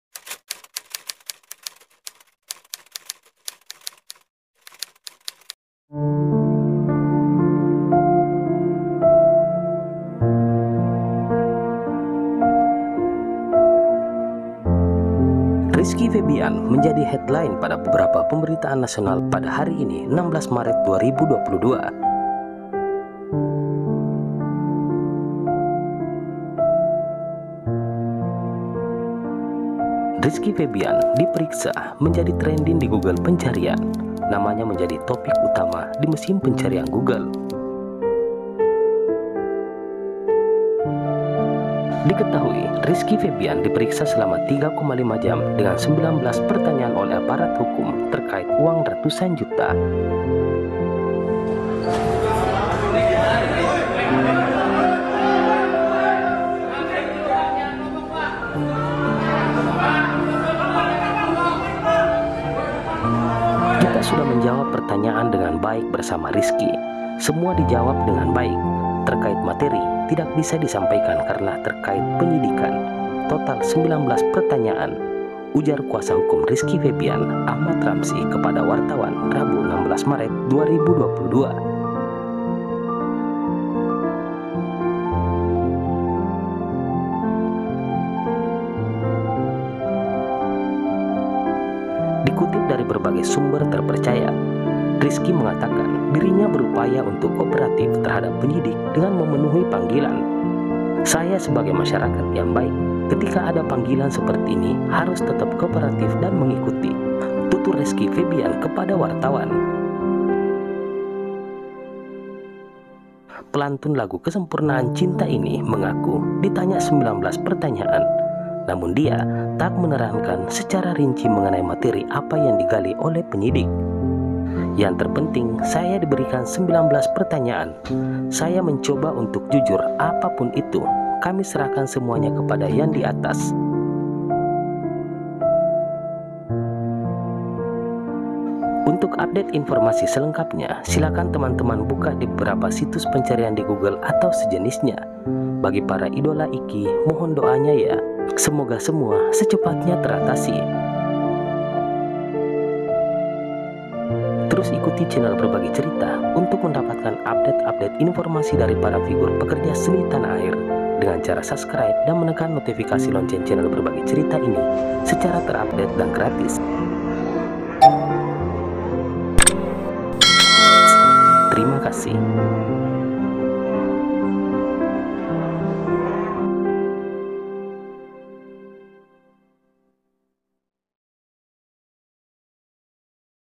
😔 RIZKY FEBIAN DIPERIKSA 3,5 JAM DENGAN 19 PERTANYAAN| MOHON DOANYA🙏🥺
Rizky Febian menjadi headline pada beberapa pemberitaan nasional pada hari ini 16 Maret 2022.

Rizky Febian diperiksa, menjadi  tranding digoogle pencarian. Namanya menjadi topik utama di mesin pencarian google.

Diketahui Rizky Febian periksa selama 3,5 jam dengan 19 pertanyaan oleh aparat hukum terkait uang ratusan juta.

Tetap sabar ya @RizkyFebianOfficial semua ujian pasti ada jalan keluarnya, Amin

Satukan doa @mahaliniraharjaofc @PutriDelinaProductions @OFFICIALSLMEDIA @mylinzofficial3058